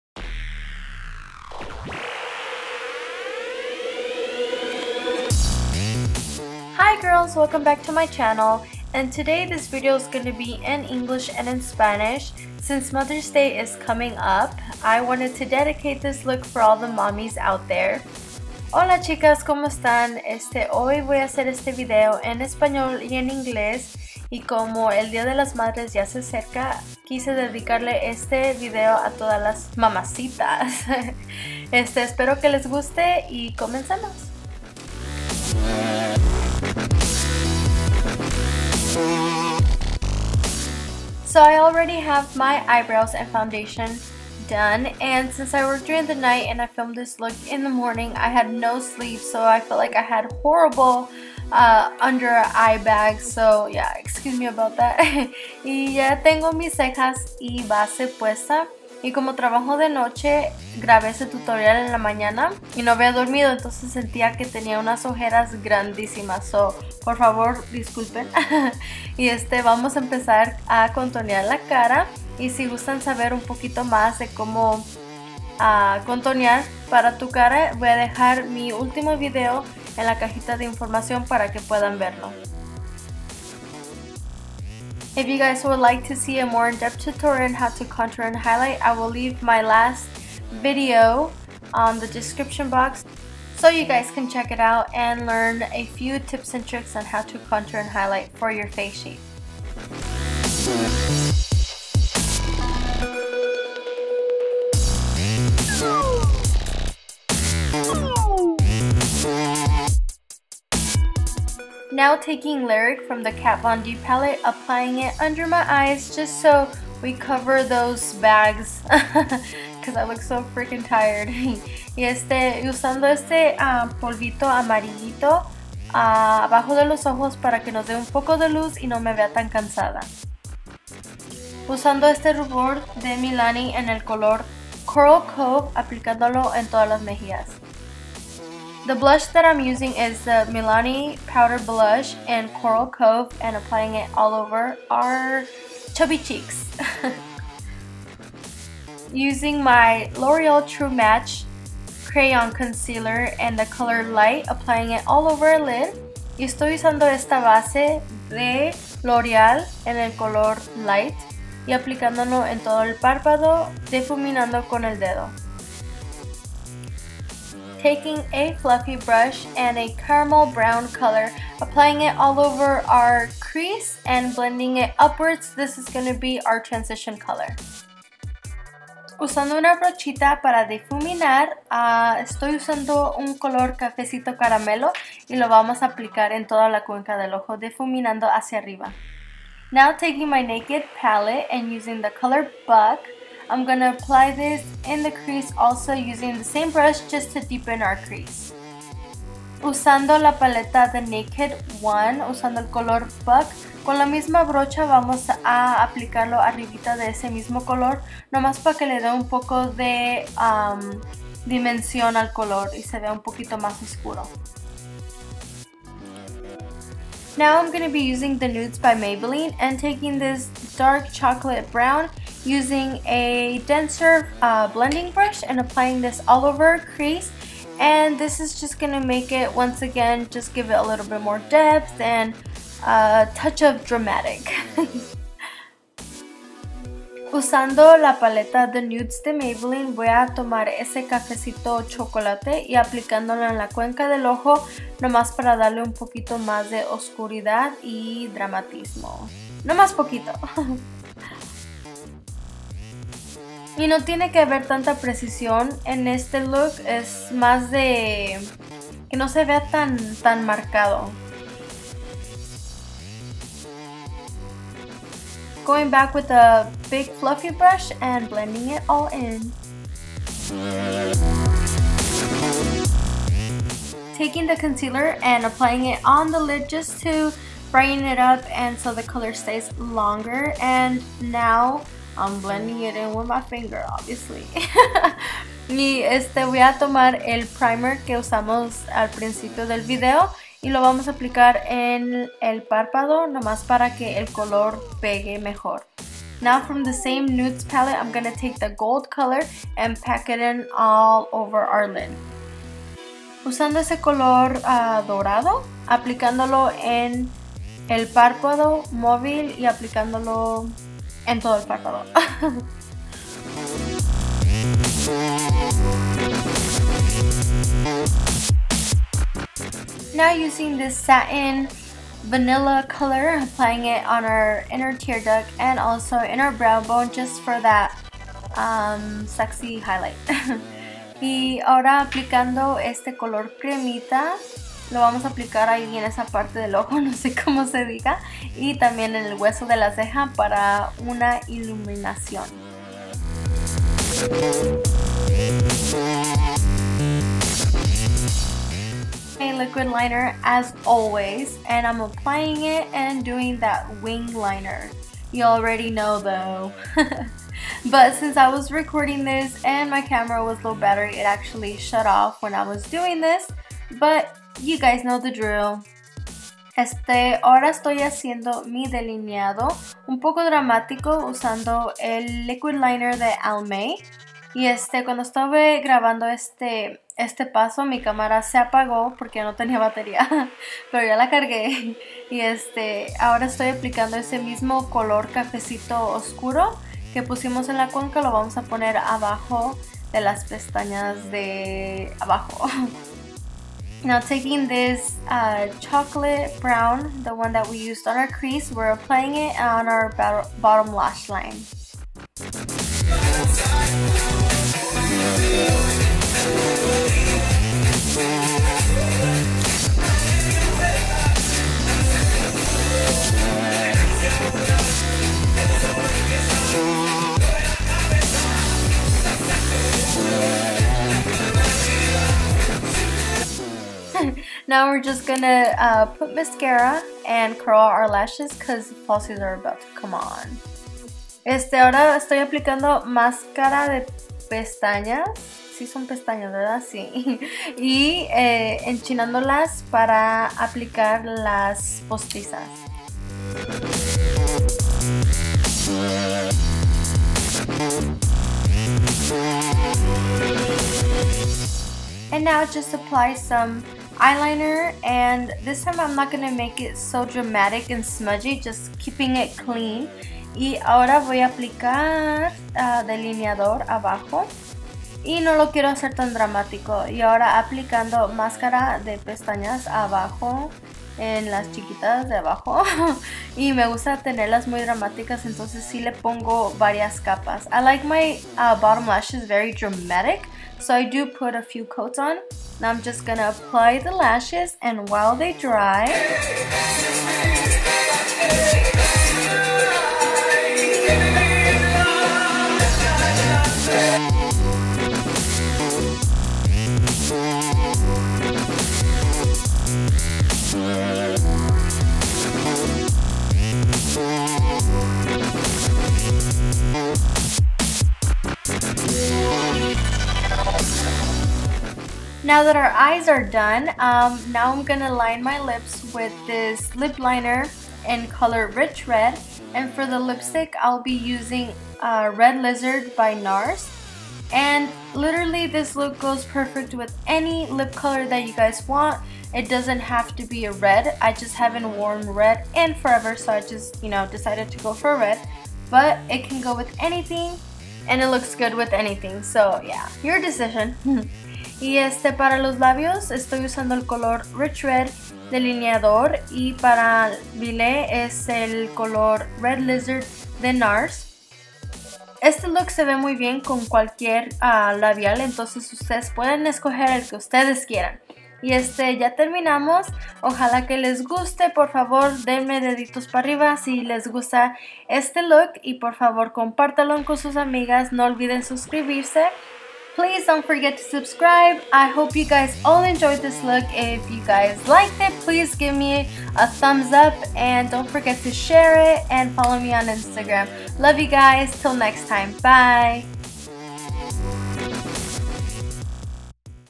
Sultry Eyes + Red Lip - ENGLISH & SPANISH
WELCOME BACK VIXXEN DOLLS!!!
i hope you all enjoyed this look and the struggle of me talking in spanish!!